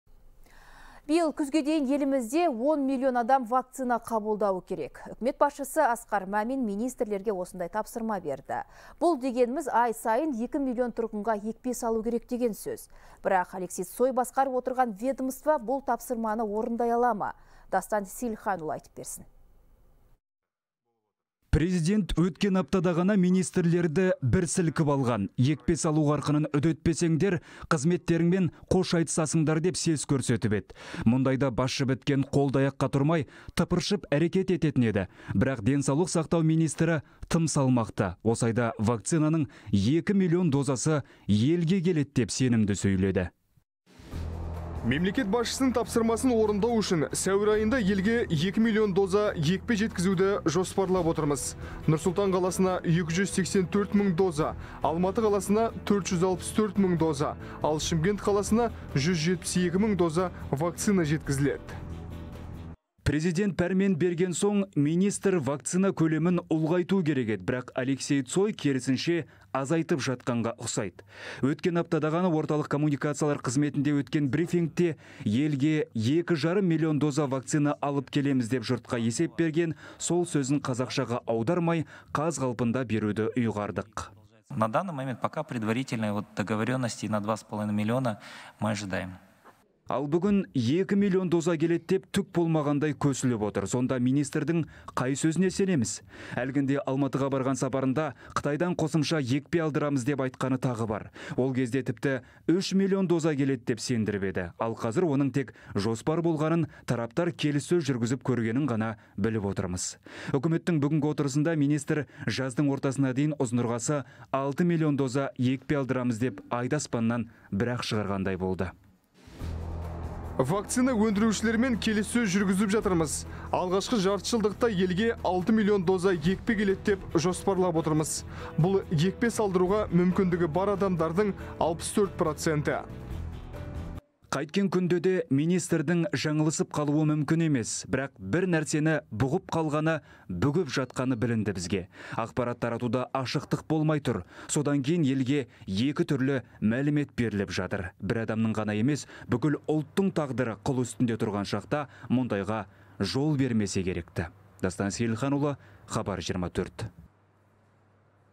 Министр сенімді ақтай ала ма?
Биыл күзге дейін елімізде 10 млн адам вакцина қабылдауы керек.